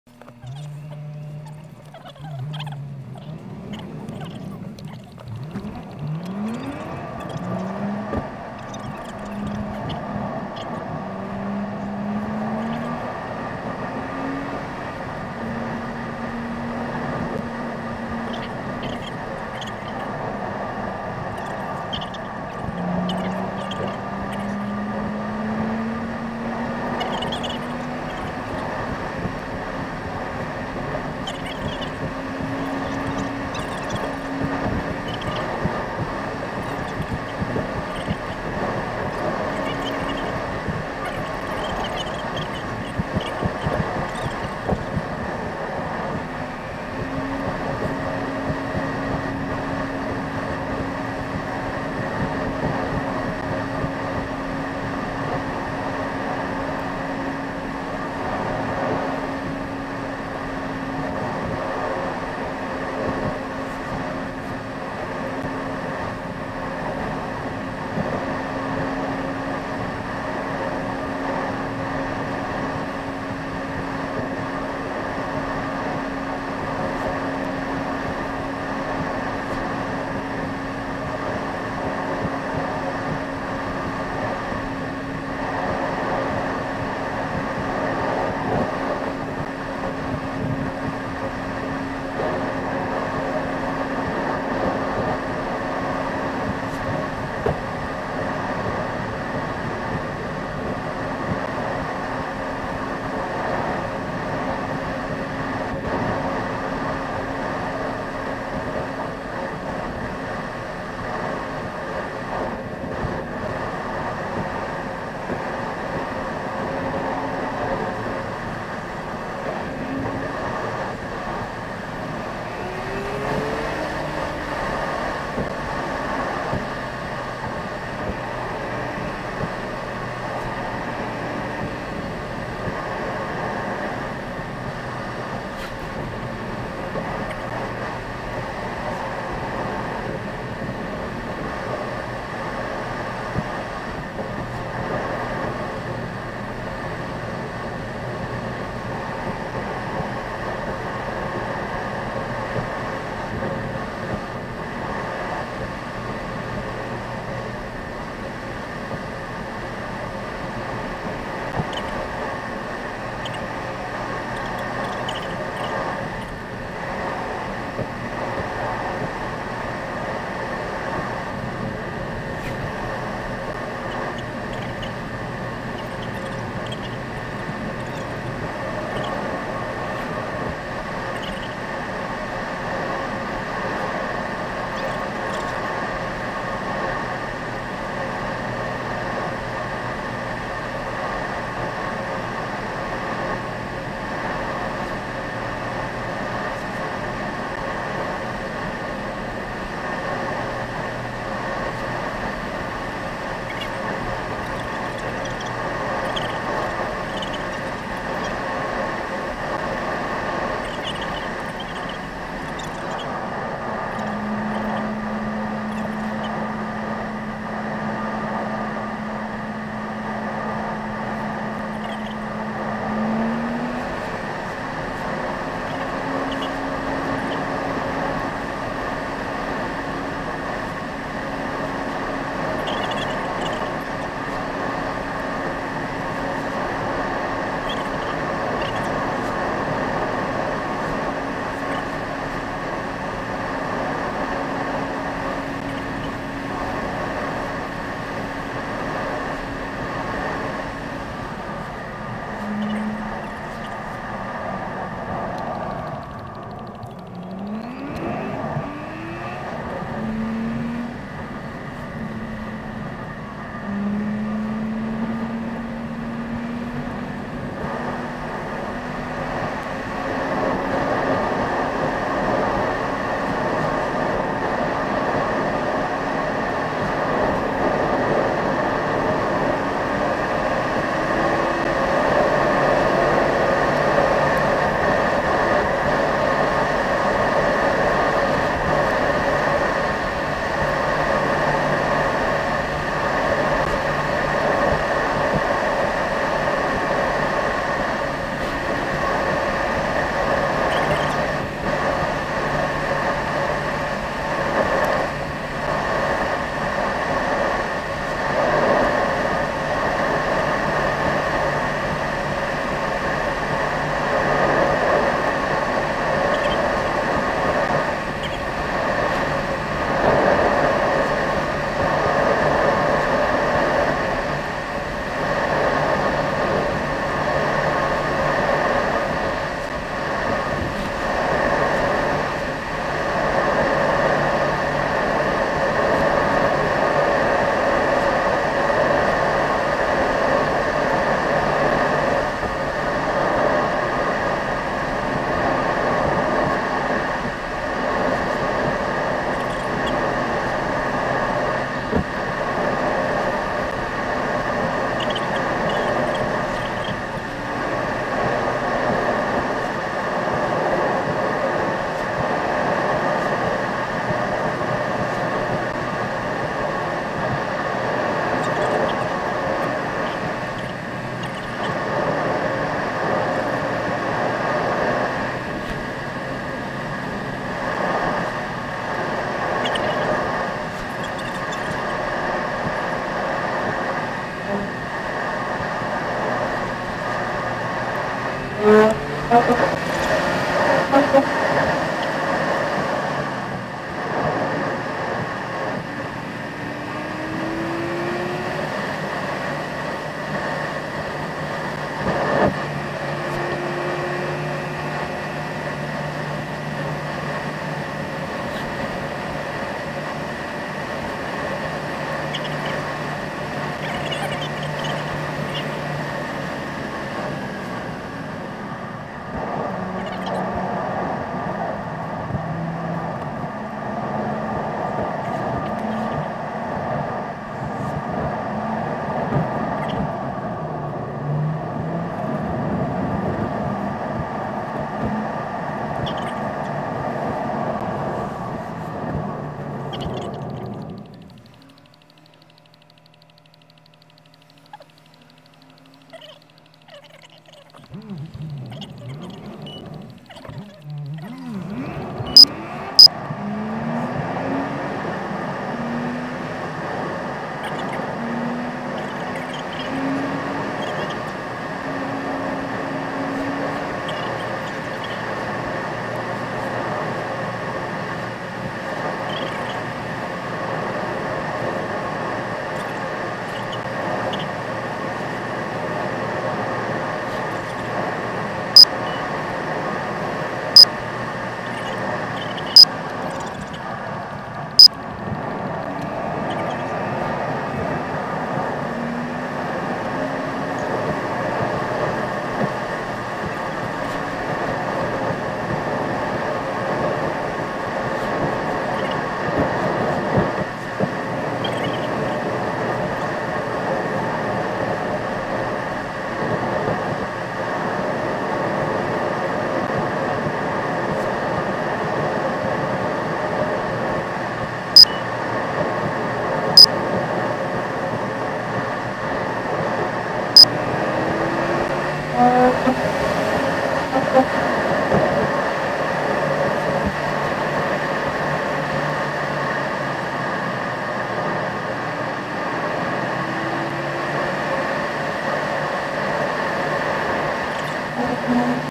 05 2018 Nummijärven kirkko Hannuksela via Seinäjoki
Tampereelta Seinäjoelle

Nopeutettu kojelautavideo
Garmin Dash Cam 55,